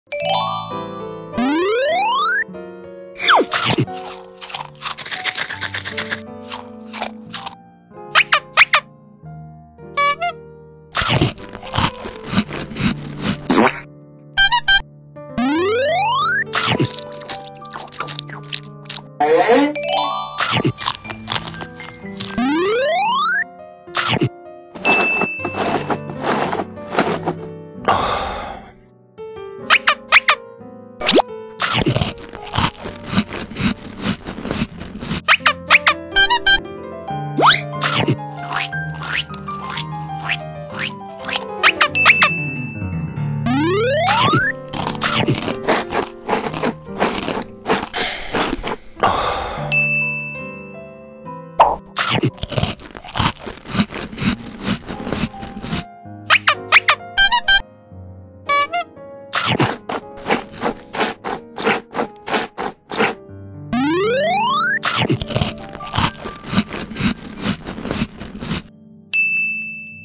Battery Eat Icecream Mukbang Animation | Battery Mukbang Animation #32
MOM VS DAD Low Battery Eat Icecream - Eating Sounds Mukbang - Smart Fast Charger Animation

Animated By : Smart Fast Charger Animation
Vector : Designed by  pch.vector/Freepik , Smart Fast Charger Animation